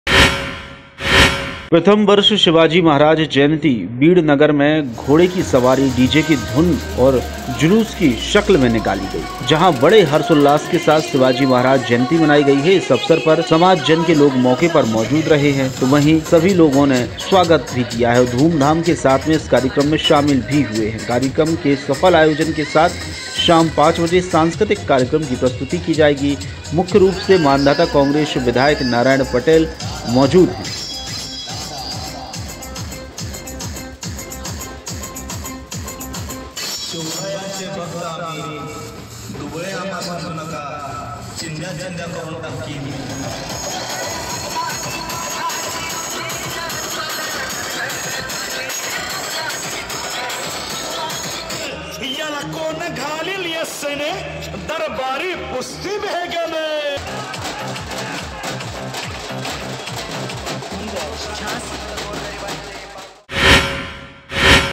NEWS खंडवा शिवाजी महाराज जयंती पर जुलुस निकाला
खंडवा
शिवाजी महाराज जयंती पर जुलुस निकाला